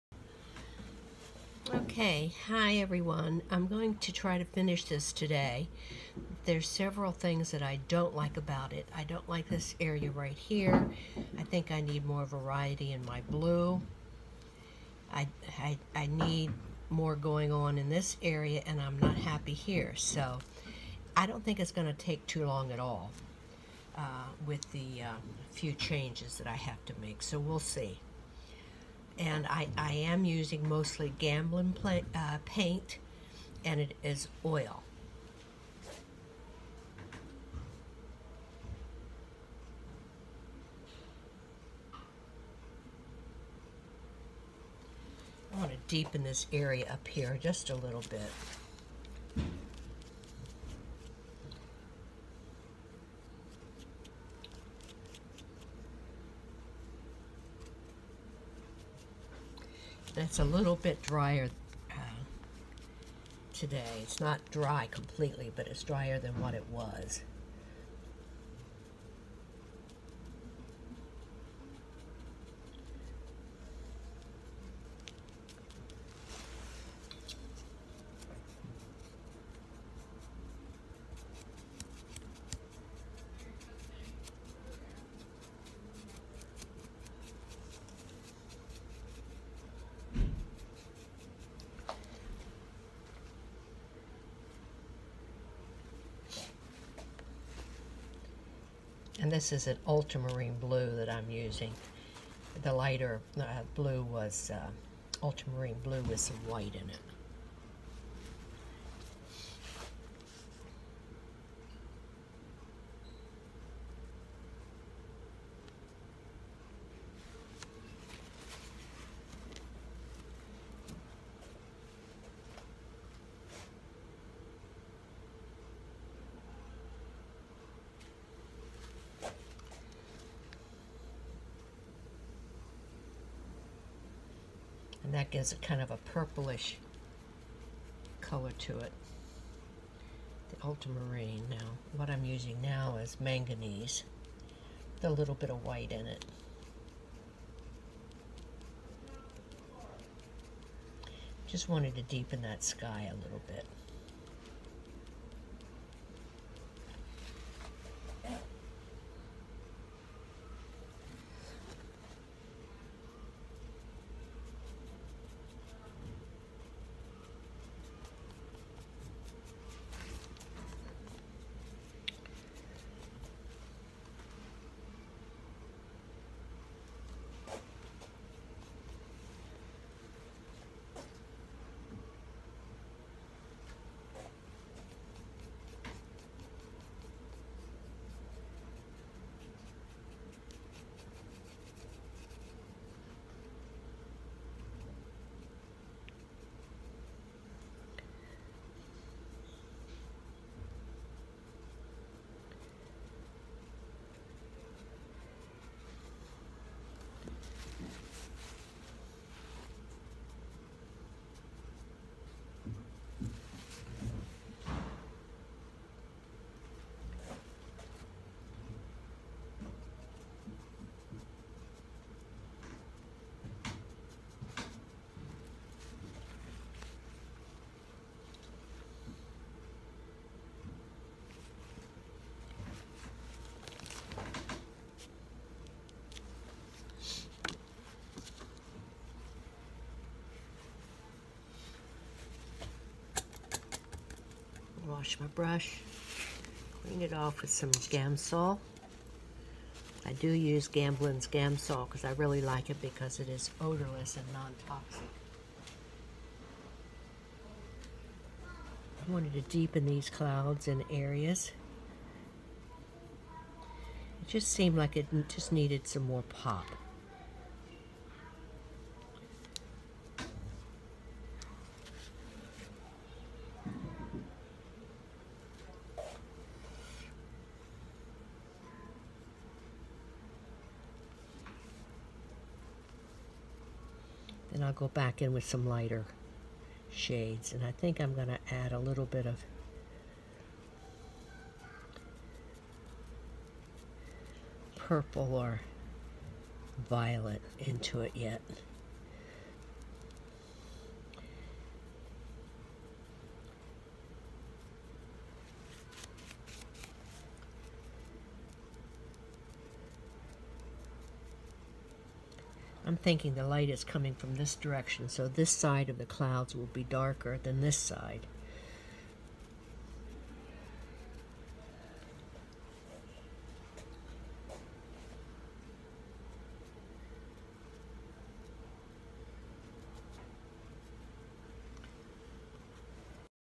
#3 - Oil Cloudscape Finish 2 Of 2 Linda Benton McCloskey 9.5.18
The finish of the cloudscape. Video interrupted, please see my Facebook page for photo of finished painting.